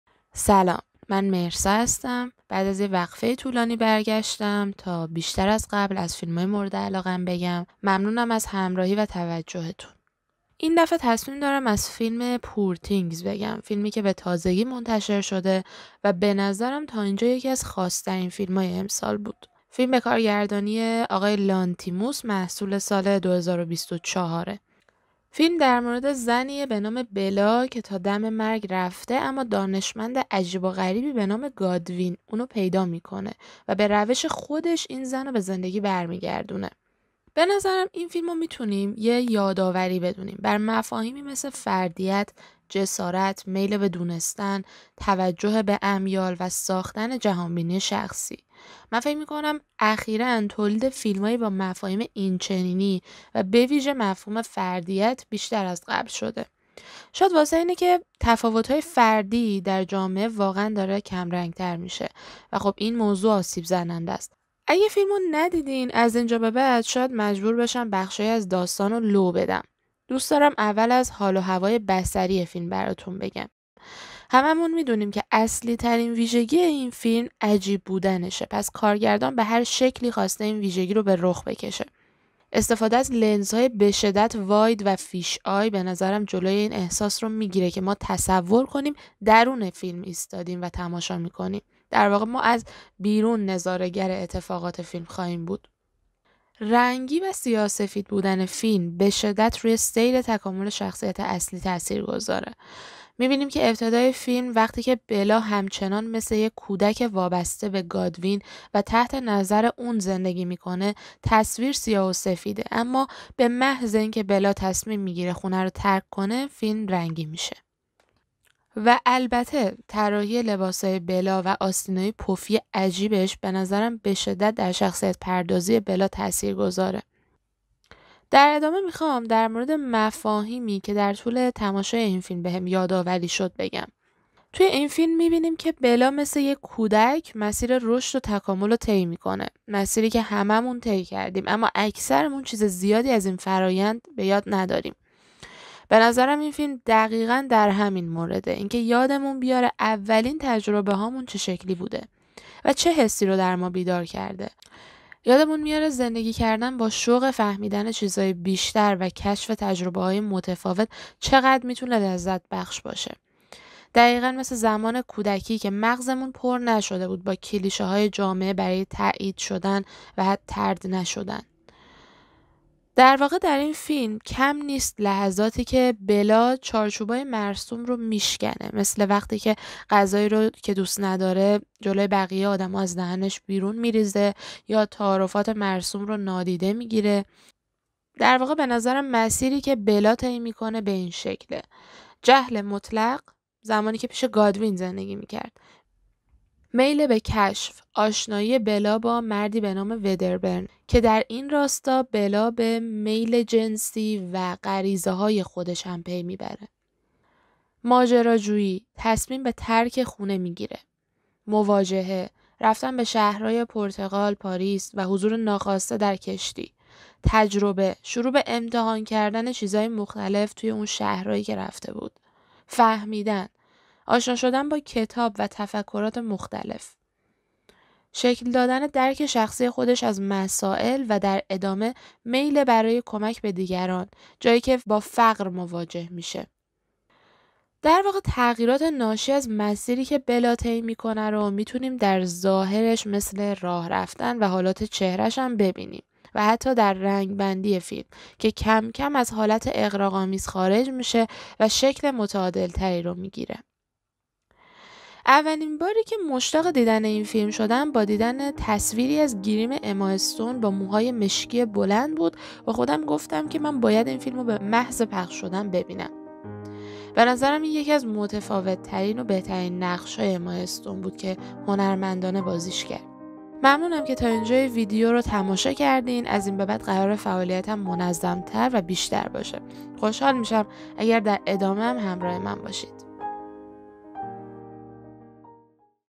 بررسی فیلم Poor things | فیلم بیچارگان
سلااام
فیلم بیچارگان جدیدترین فیلم جناب لانتیموس، بنظرم از خاص‌تربن فیلم‌های اخیر بود که حرف‌های زیادی برای گفتن داشت و من تصمیم گرفتم بیام نظر و دیدگاهمو بگم.
و خیلی خوشحال میشم اگر شما هم ایده و نظراتتون رو مطرح کنید تا بتونیم بیشتر از فیلما بگیم و بدونیم.
امید که ماندگار باشید.